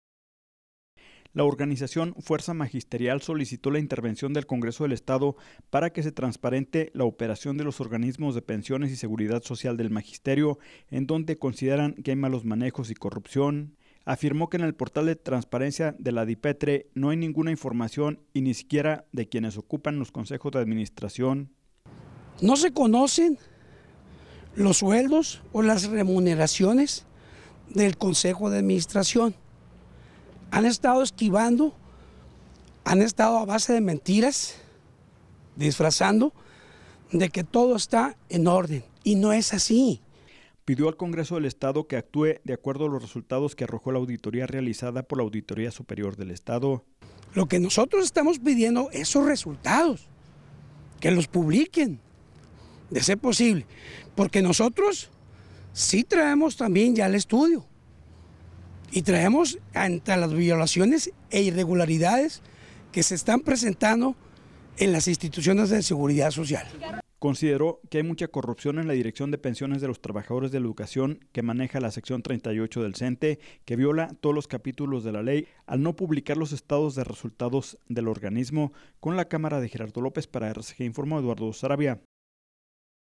Piden al Congreso clarificar funcionamiento de pensiones de la sección 38 del SNTE
La organización Fuerza Magisterial, solicitó la intervención del Congreso del estado para que se transparente la operación de los organismos de Pensiones y Seguridad Social del magisterio, en donde consideran que hay malos manejos y corrupción.

Afirmó que en el portal de transparencia de la DIPETRE no hay ninguna información, ni siquiera de quienes ocupan el Consejo de Administración.